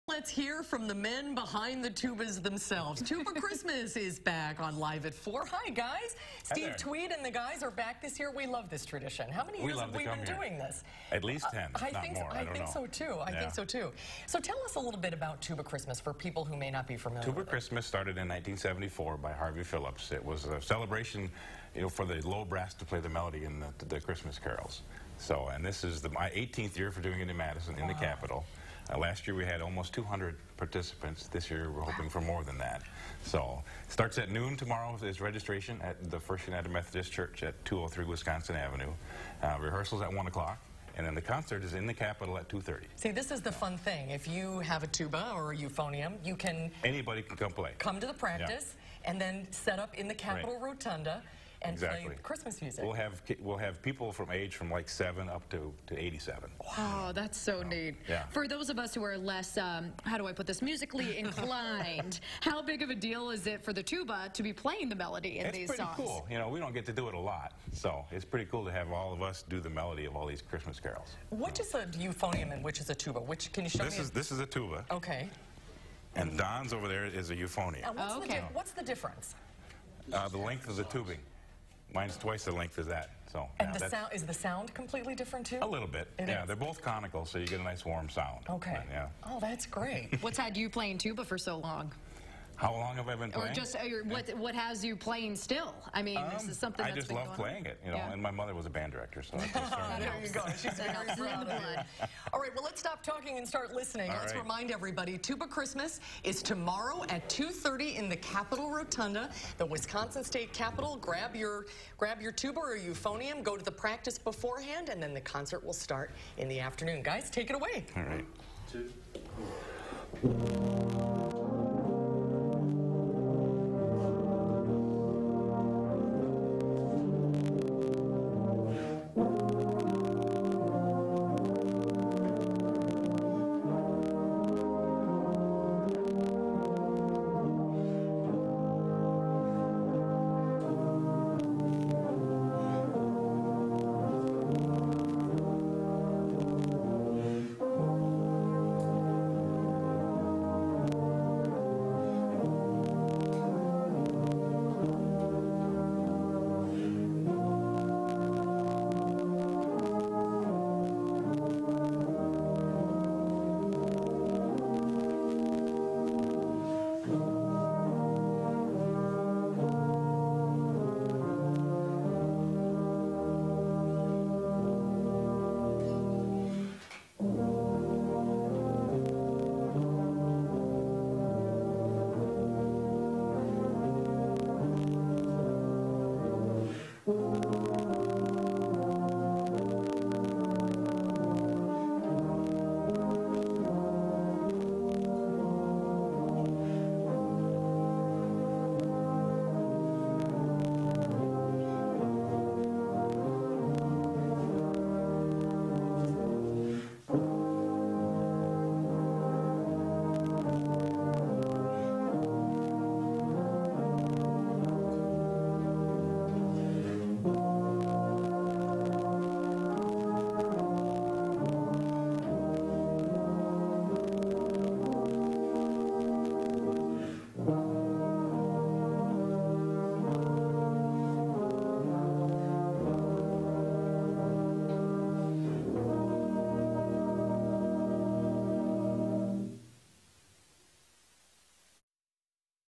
2019 Madison Tuba Christmas
You've had the chance to hear them a few times during the show already, but now let's hear from the men behind the tubas!